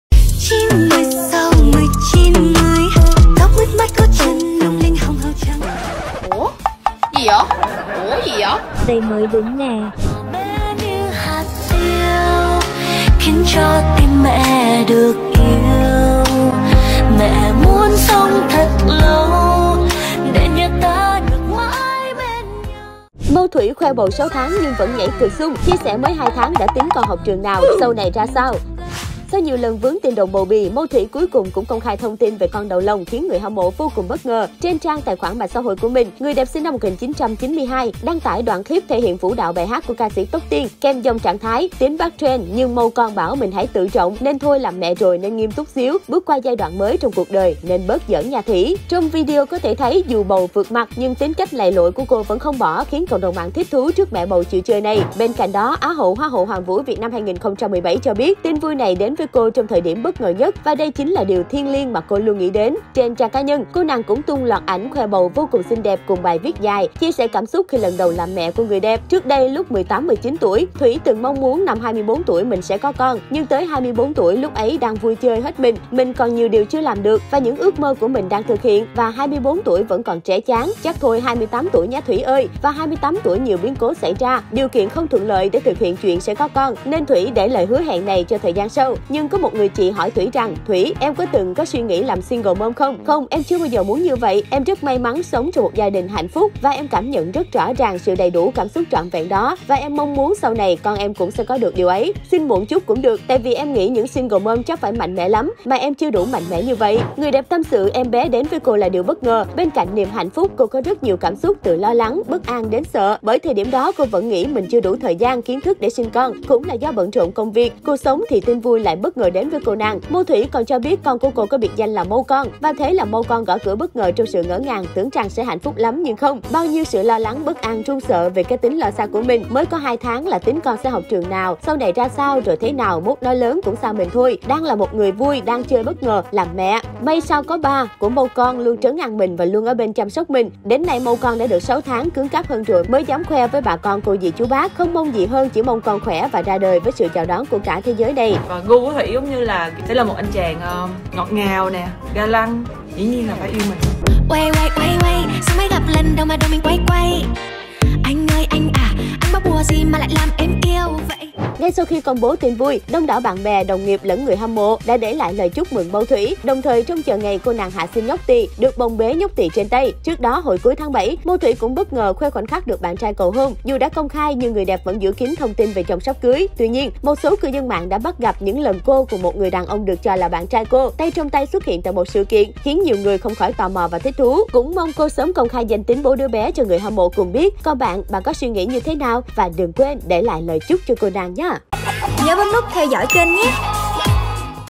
Mâu Thủy xác nhận MANG THAI 6 tháng sau nhiều đồn đoán: Danh tính chú rể được CĐM săn đón
Là một Á hậu, người mẫu nổi tiếng trong làng nhan sắc Việt, Mâu Thủy từ sau công khai hình ảnh được bạn trai cầu hôn càng nhận về nhiều sự quan tâm của người hâm mộ. Sau nhiều lần vướng tin đồn bầu bì, người đẹp cuối cùng cũng công khai thông tin về con đầu lòng khiến công chúng vô cùng bất ngờ.

Hãy theo dõi Yan trên:
✔️Youtube Yan News :